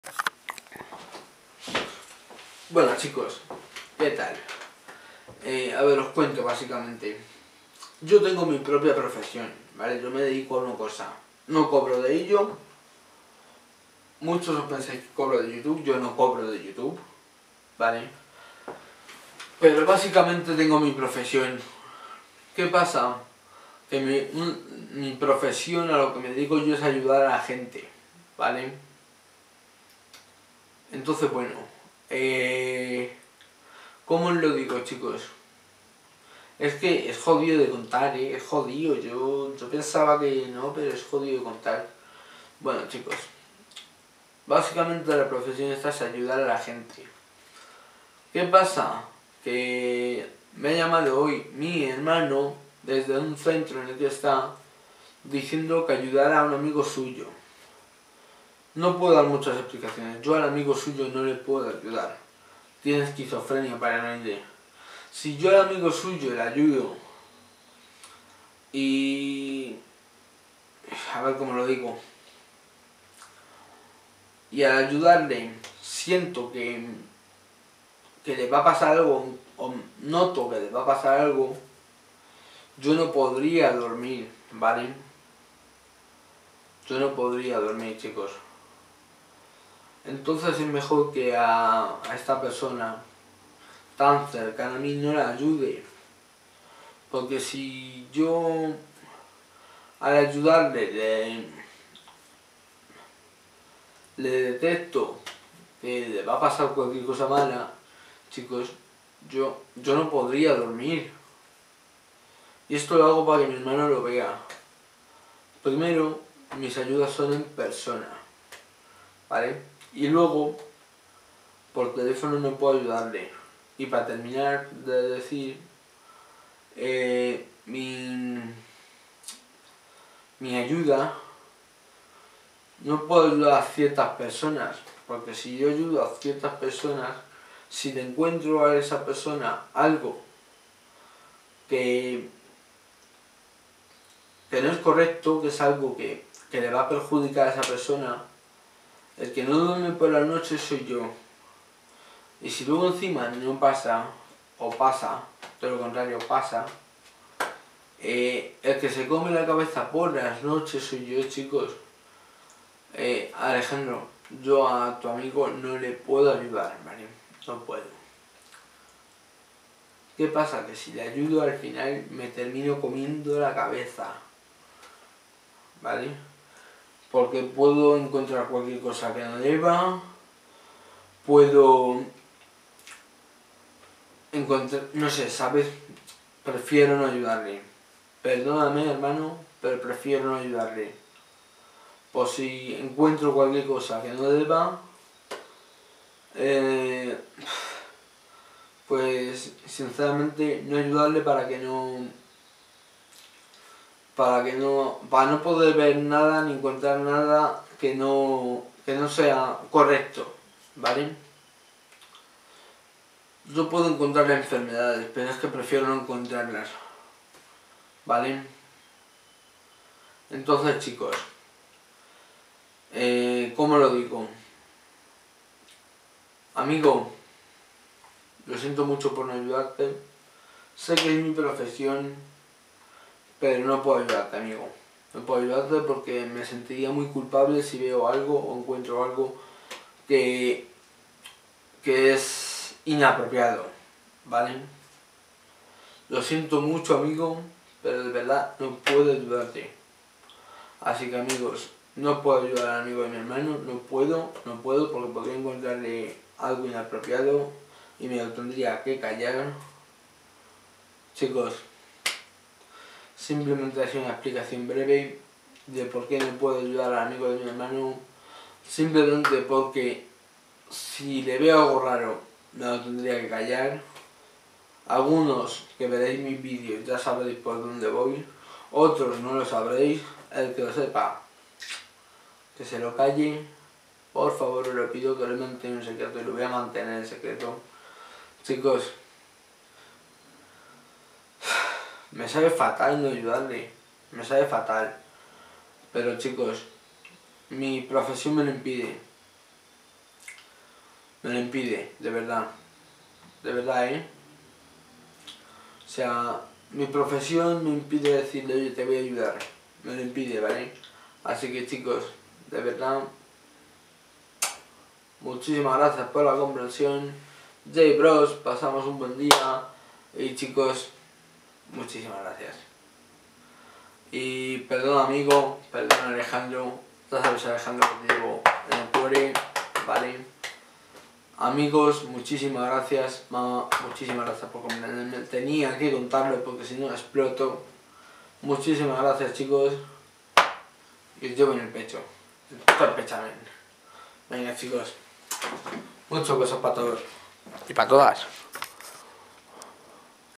Y POR QUE ME PIDES ESTO? MI PROFESION 💙
Me he quedado sin palabras al venir un familiar y pedirme que le ayude a un amigo suyo con un problema de su amigo, si sabe el que yo no puedo ayudar a cualquier persona, LO SIENTO AMIGO, no puedo ayudarte